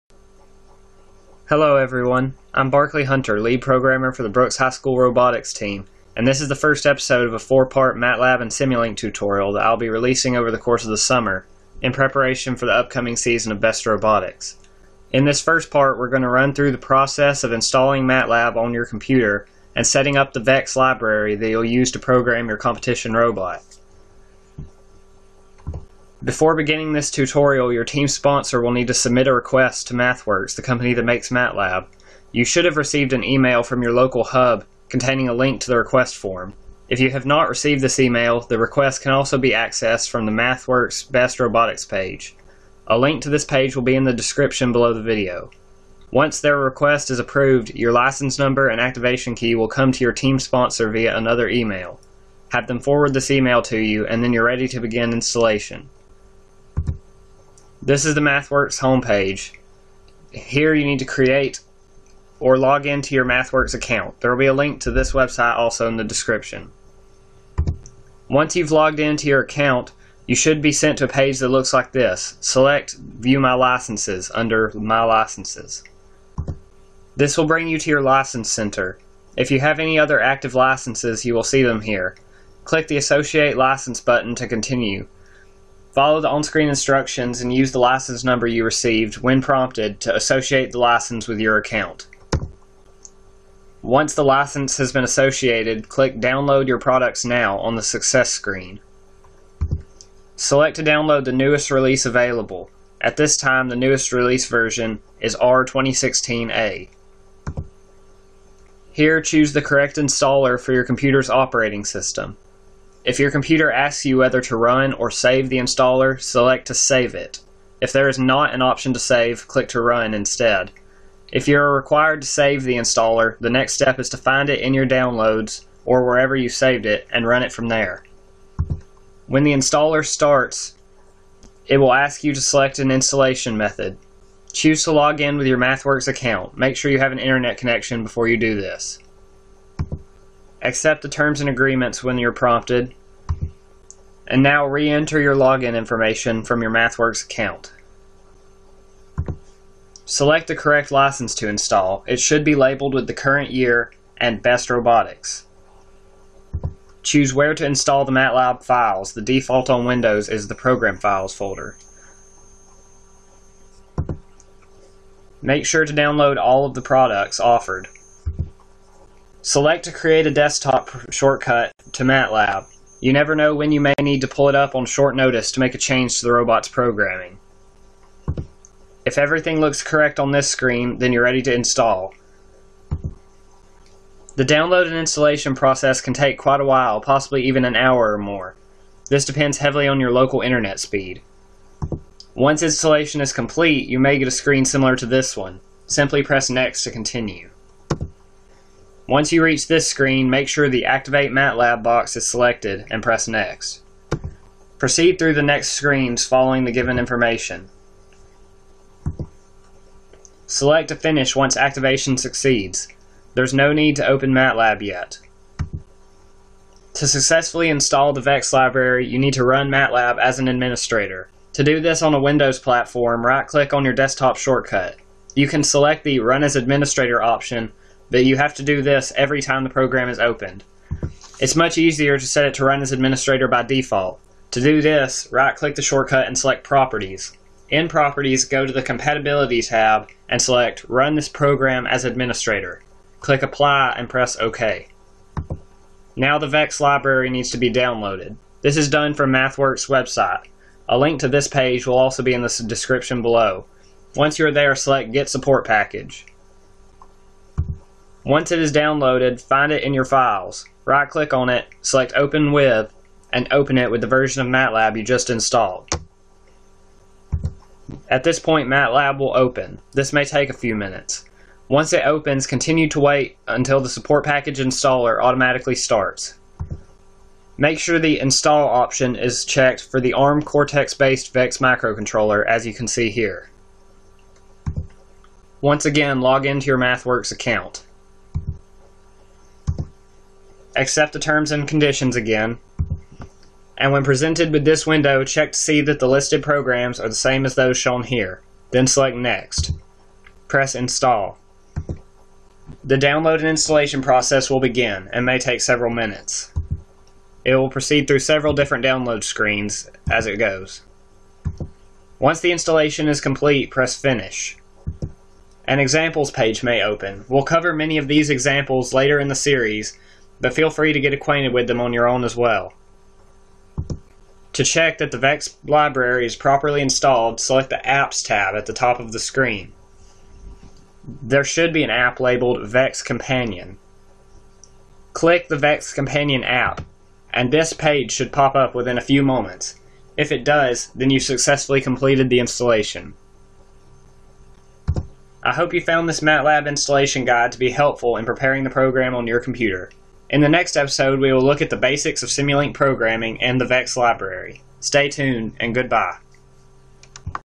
BEST Robotics MATLAB & Simulink Tutorial Part 1: Installation and Setup
In this first episode of a four-part series, we will look at the process of downloading and installing the software necessary for using MATLAB in the BEST Robotics competition.